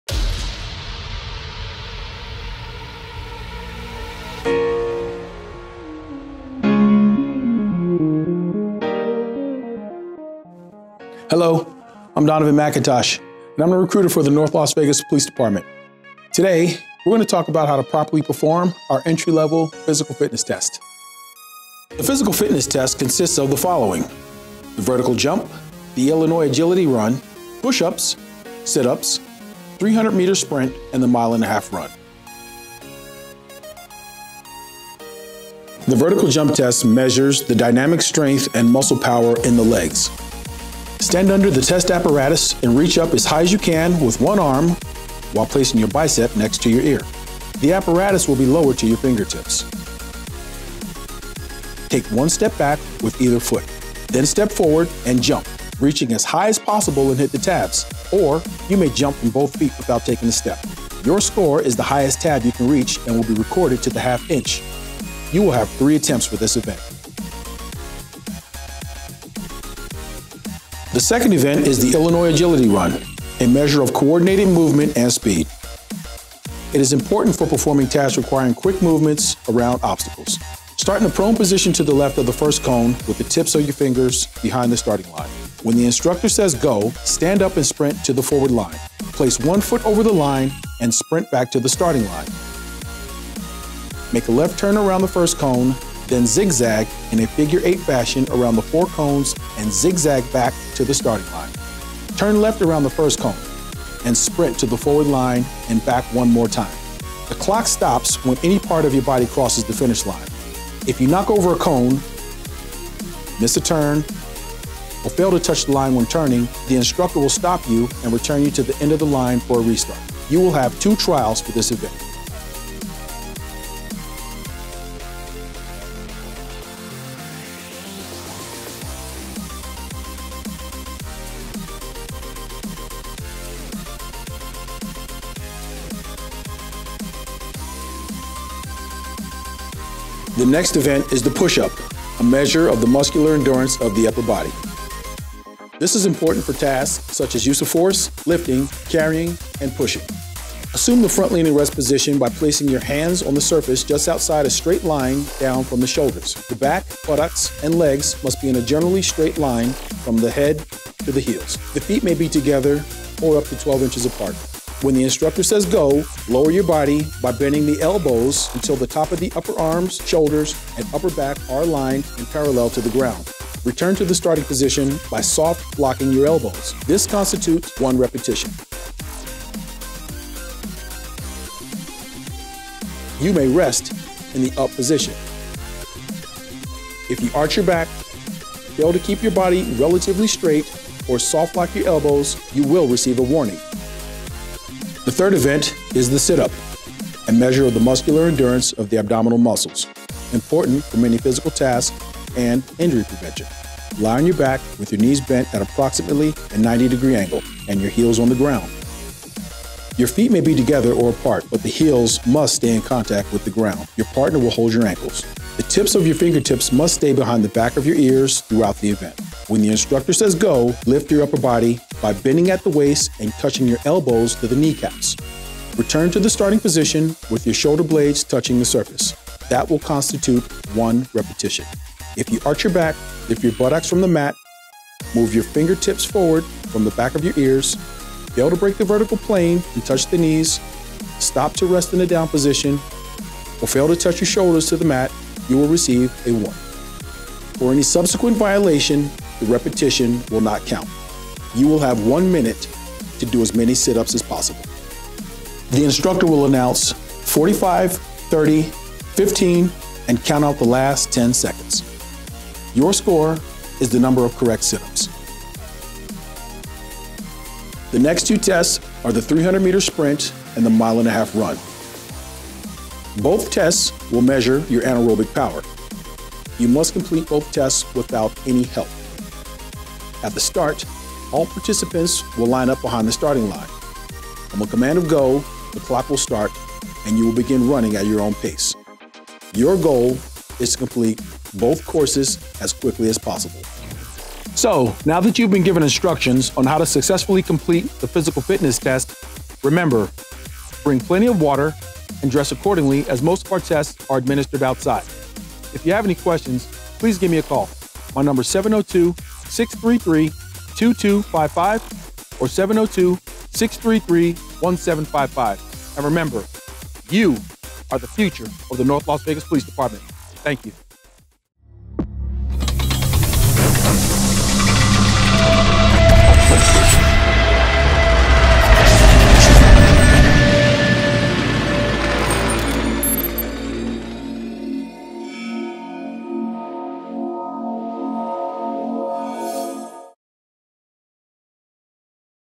PFT Instruction Video
All the requirements for the North Las Vegas Physical Fitness Test